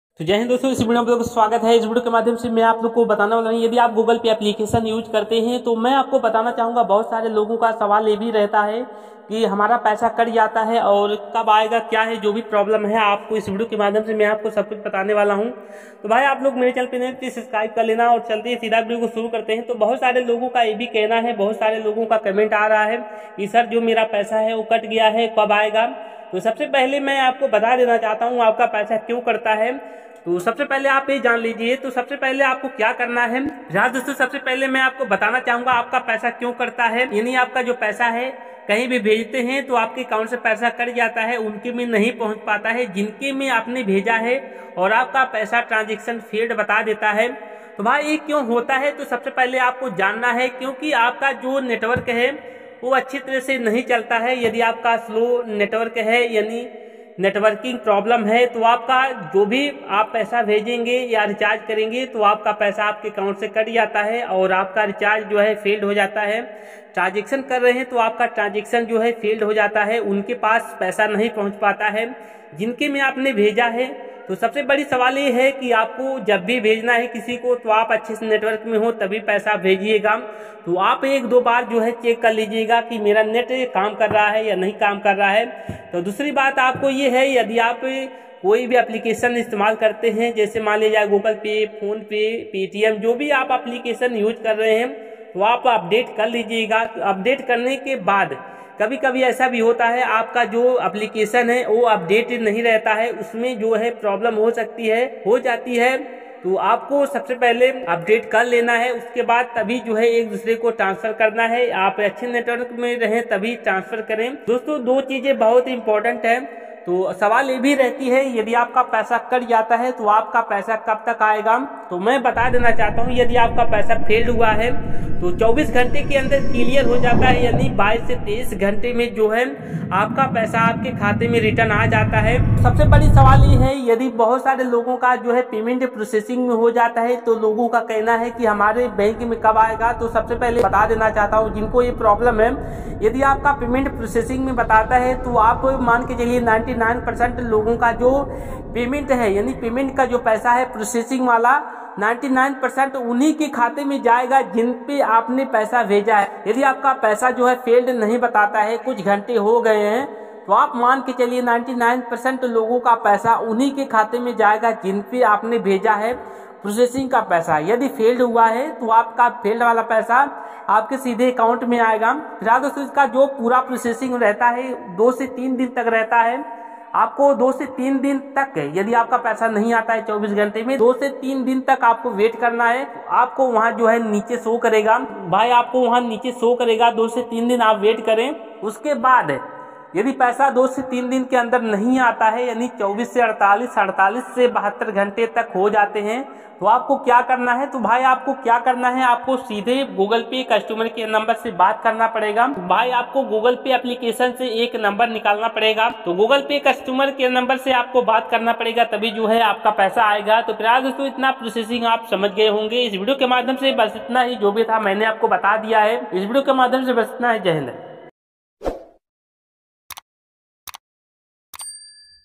Google pay recharge fieled । Google pay transaction processing failed । पैसा कब आएगा जाने 2024
Google pay se Paisa cut Gaya vapas kaise magayen
Google pay recharge field problem